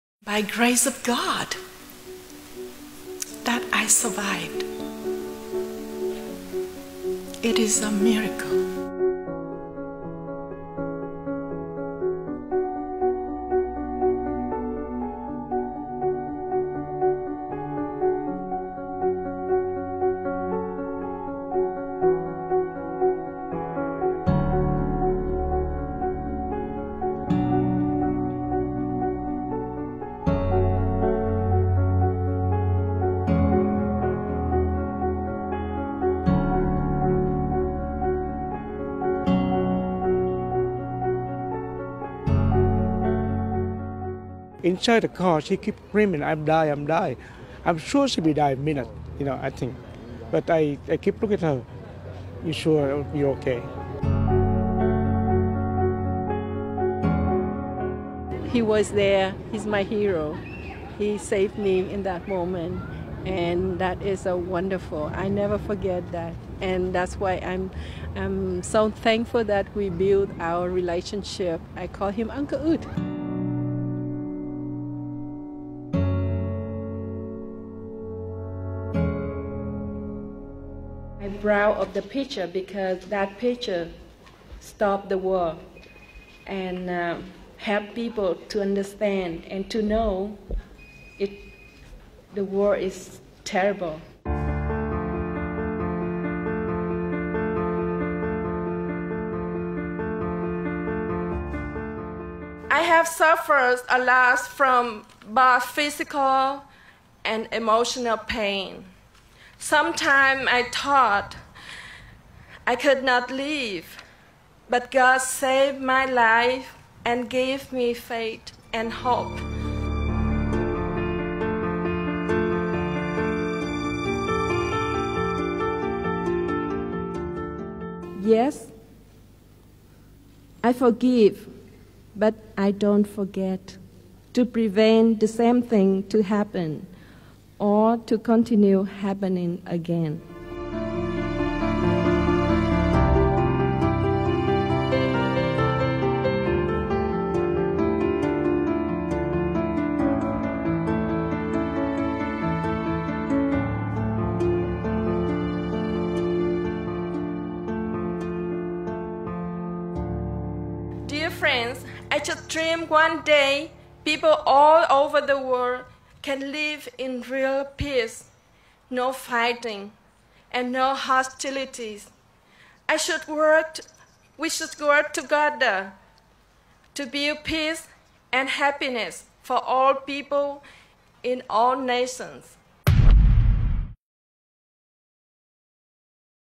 The Napalm Girl at the Vietnam War
The Napalm Girl photo changed the way the world looked at the Vietnam War